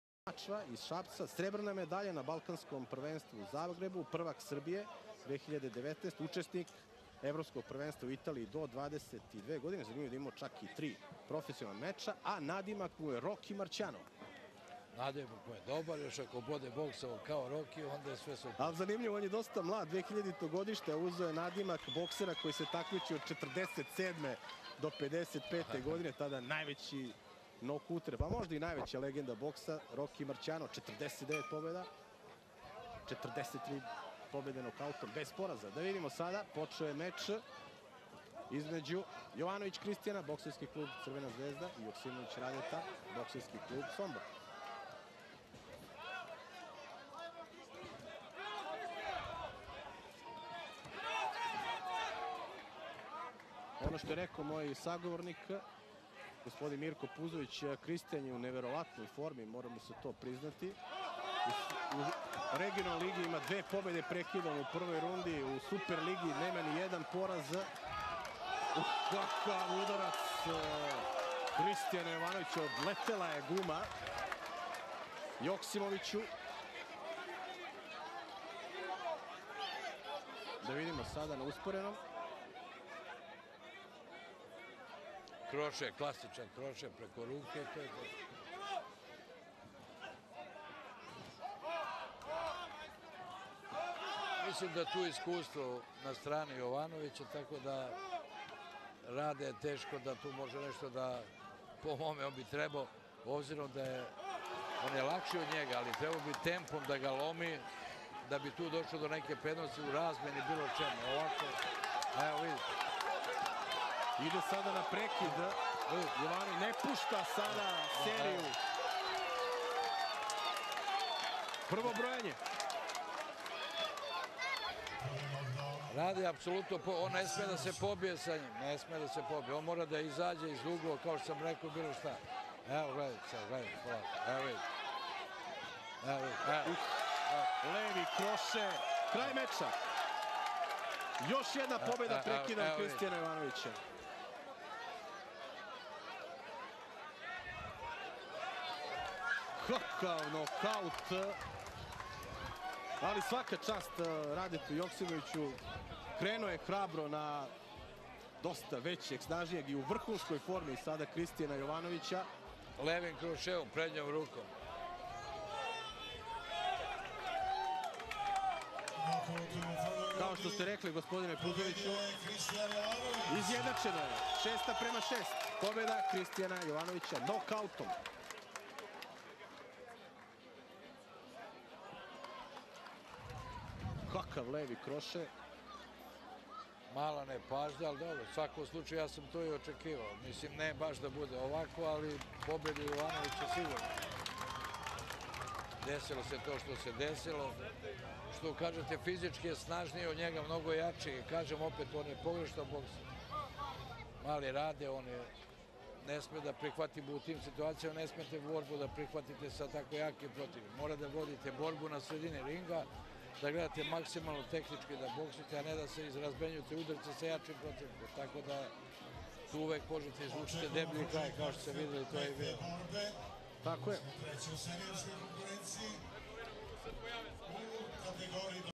Jovanovic Kiki Kristijan vs Joksimovic Rade 57kg Super liga Srbije u Boksu 3.mec /Finale
Preuzeto sa Arena Fight
AUTORSKA PRAVA ARENA Tv